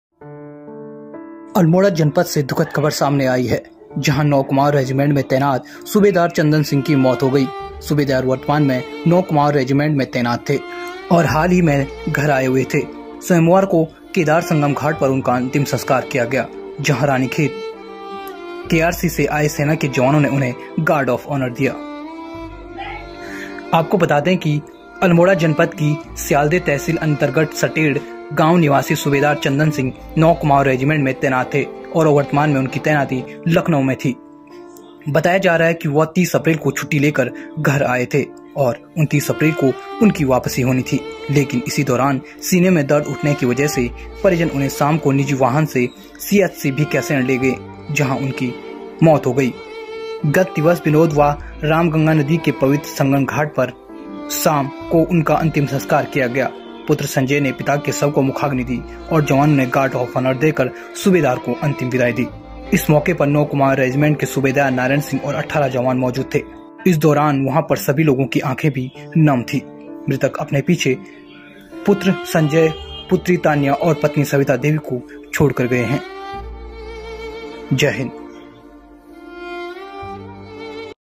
छुट्टी पर घर आया था सेना का जवान तिरंगे में लिपटकर चला गया || Subedar Chandan Singh || Uttarakhand
इन दिनों सूबेदार चंदन सिंह की ड्यूटी लखनऊ में थी। 30 अप्रैल को वो छुट्टी लेकर घर आए थे, लेकिन किसे पता था कि ये छुट्टियां उनके जीवन की आखिरी छुट्टियां साबित होंगी। भावभीनी विदाई